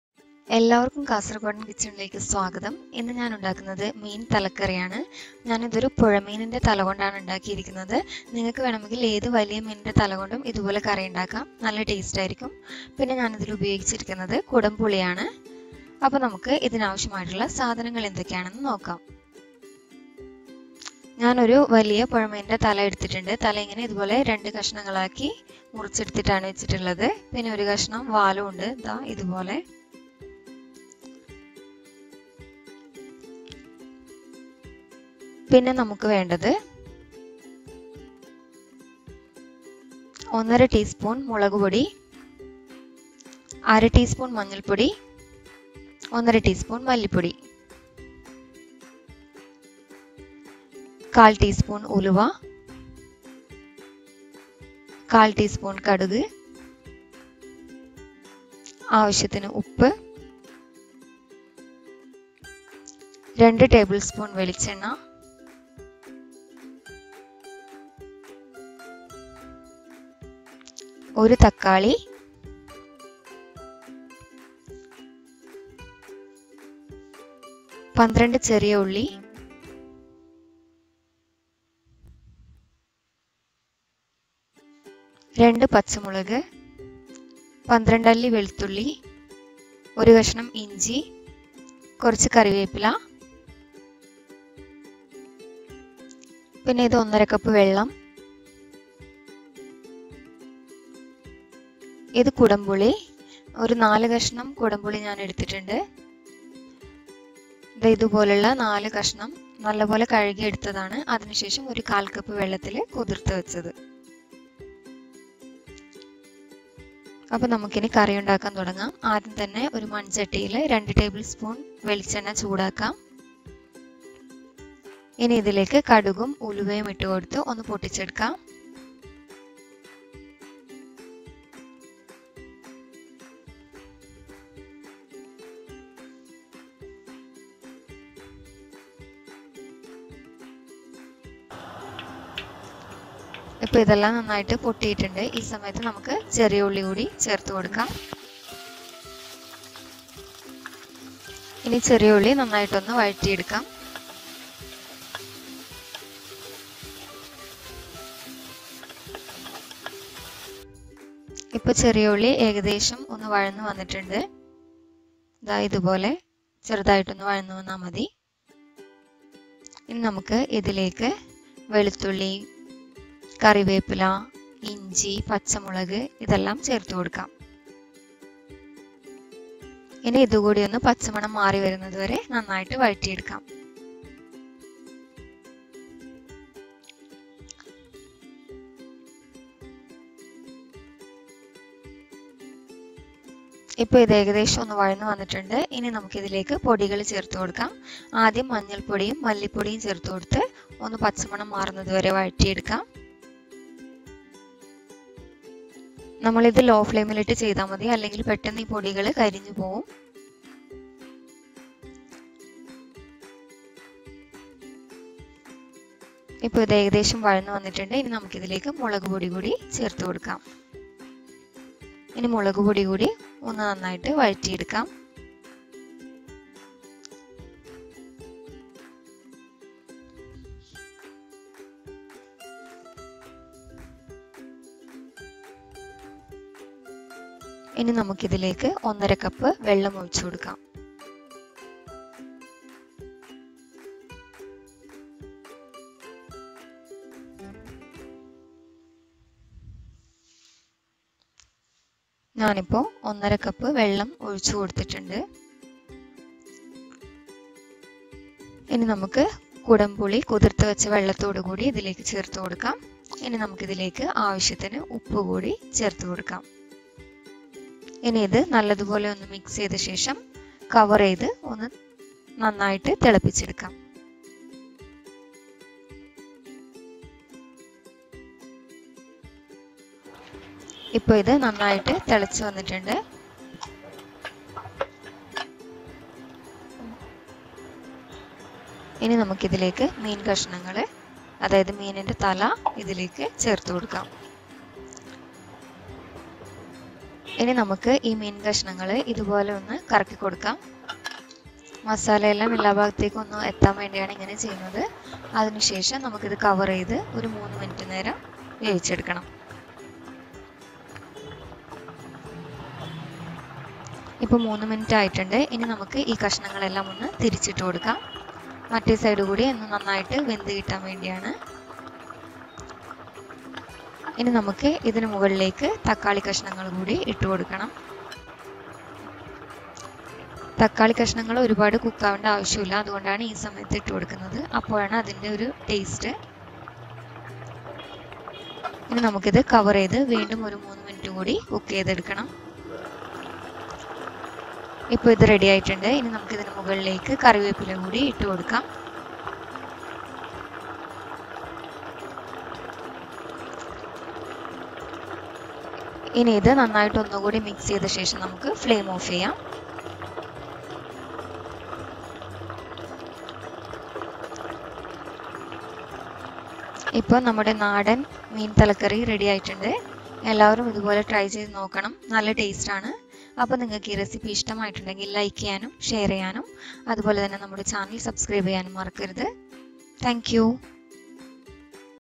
നാടൻ മീൻ തല കറി | Kerala Meen Thala Curry | Fish Curry Recipe
Kerala Fish Head Curry Recipe in Malayalam.

Ingredients

Fish head - 1
Malabar tamarind - 4 pieces
Shallots - 12
Garlic - 12
Tomato - 1
Curryleaves
Salt